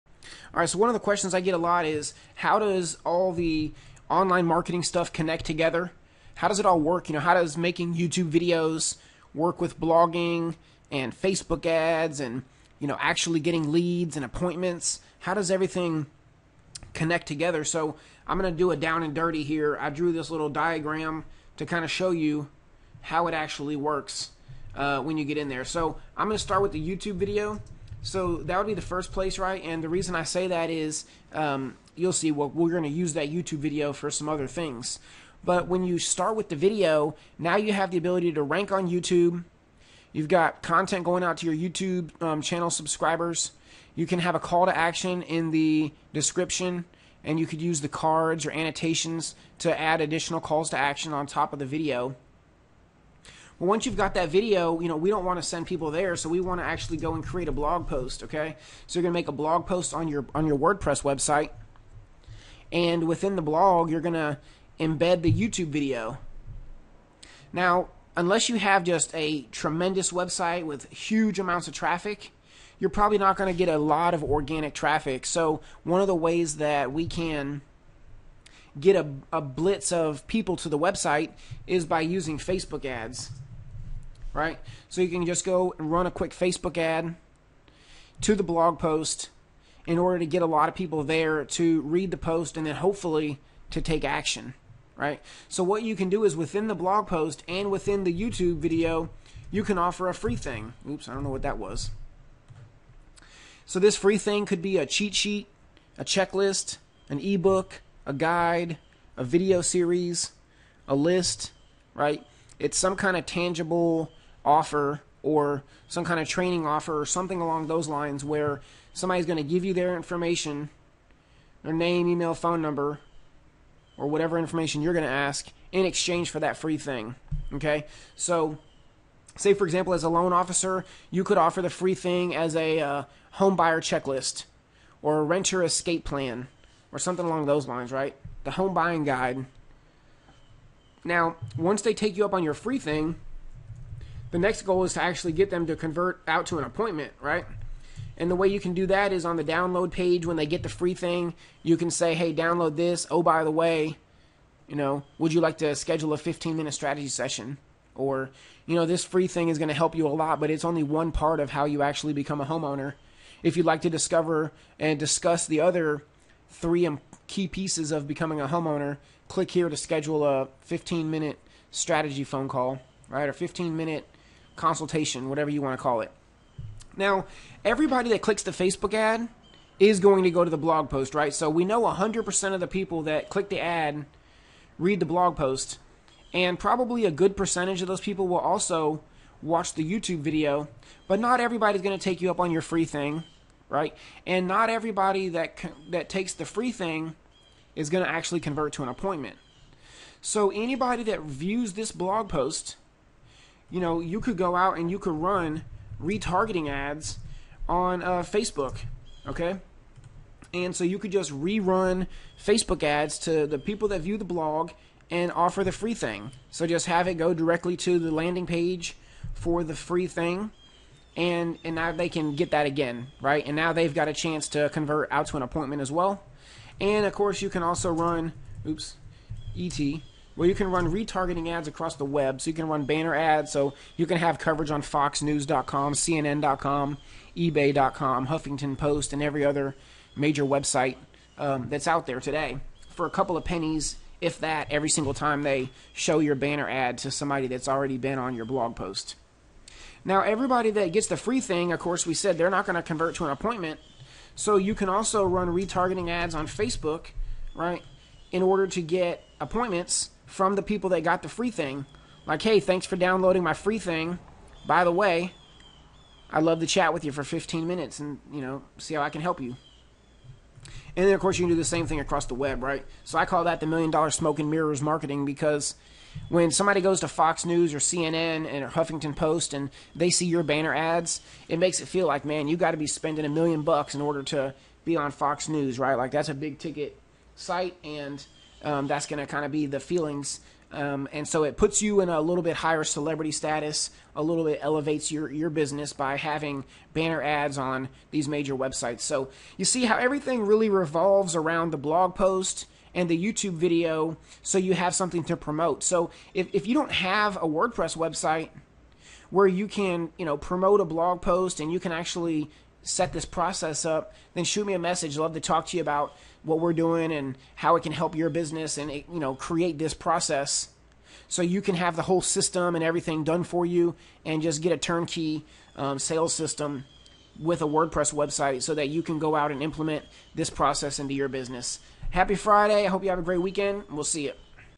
How Loan Officer Online Marketing All Connects Together
Do you want to figure out how all this loan officer online marketing stuff connects together to make you a local celebrity, get more leads, and grow your mortgage business?

This 5 minute tutorial covers how to use Facebook, a Wordpress blog and YouTube to get leads flowing into your business.

Need help with this?